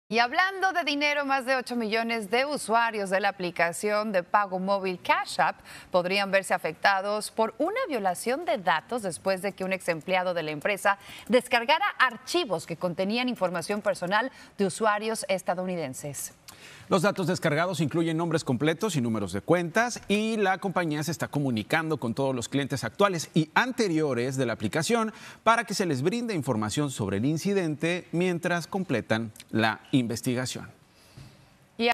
Exempleado de Cash App bajó datos de 8 millones de usuarios | Noticias Telemundo
Video oficial de Noticias Telemundo. La información descargada incluye nombres completos y números de cuenta de los usuarios. La compañía se está comunicando con todos los clientes para brindarles más información sobre el incidente mientras se realiza una investigación.